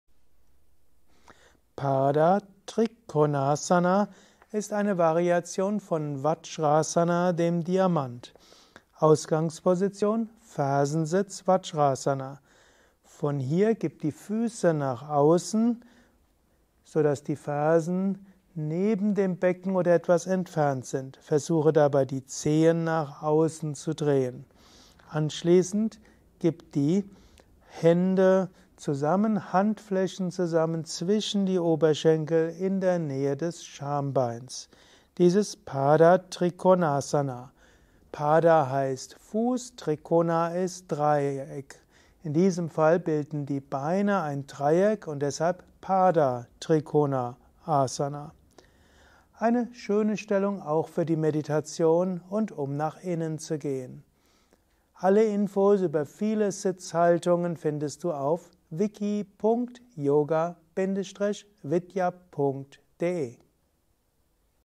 Pada Trikonasana - Asana Lexikon 929
Pada Trikonasana bzw. पादत्रिकोणासन, pāda-tri-koṇāsana: Erläuterung, Vorführung und Übungsanleitung. Pada Trikonasana gehört im Yoga zu den Asanas im Sitzen.

Padatrikonasana ist eine Variation von Vajrasana

Wissenschaftliche Umschrift (IAST): pāda-tri-koṇāsana